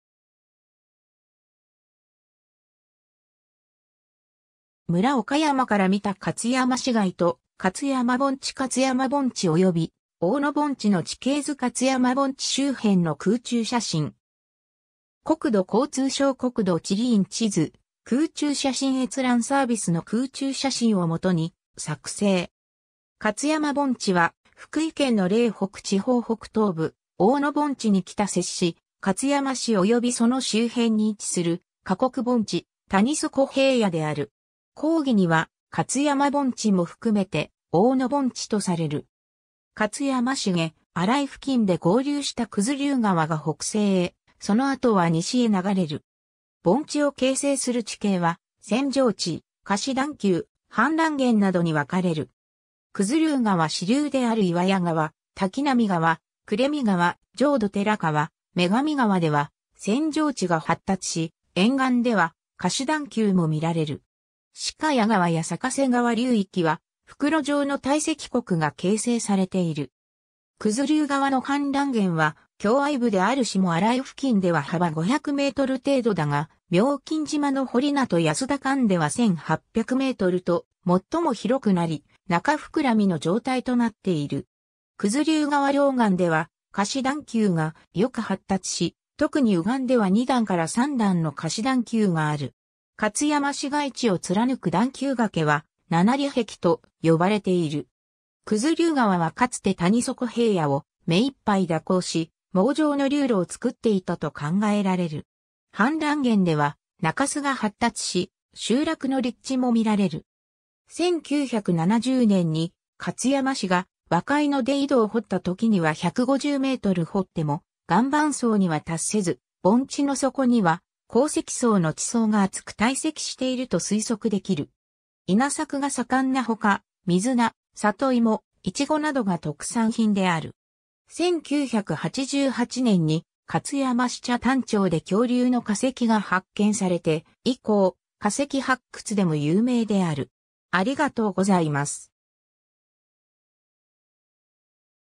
勝山盆地
村岡山から見た勝山市街と勝山盆地 勝山盆地（中央上部）および大野盆地（中央下部）の地形図 勝山盆地周辺の空中写真。
 勝山盆地（かつやまぼんち）は、福井県の嶺北地方北東部、大野盆地に北接し、勝山市およびその周辺に位置する河谷盆地、谷底平野である。
広義には勝山盆地も含めて大野盆地とされる。
 勝山市下荒井付近で合流した九頭竜川が北西へ、その後は西へ流れる。
盆地を形成する地形は、扇状地、河岸段丘、氾濫原などに分かれる。
九頭竜川支流である岩屋川、滝波川、暮見川、浄土寺川、女神川では扇状地が発達し、沿岸では河岸段丘も見られる。
鹿谷川や坂瀬川流域は袋状の堆積谷が形成されている。
 九頭竜川の氾濫原は、狭隘部である下荒井付近では幅500m程度だが、妙金島の堀名と保田間では1800mと最も広くなり、中膨らみの状態となっている。
九頭竜川両岸では河岸段丘がよく発達し、特に右岸では2段から3段の河岸段丘がある。
勝山市街地を貫く段丘崖は「七里壁」と呼ばれている。
九頭竜川はかつて谷底平野を目いっぱい蛇行し、網状の流路を作っていたと考えられる。
氾濫原では中州が発達し、集落の立地も見られる。
 1970年に勝山市が若猪野で井戸を掘った時には150m掘っても岩盤層には達せず、盆地の底には洪積層の地層が厚く堆積していると推測できる。
 稲作が盛んなほか、水菜、サトイモ、イチゴなどが特産品である。
1988年に勝山市北谷町で恐竜の化石が発見されて以降、化石発掘でも有名である。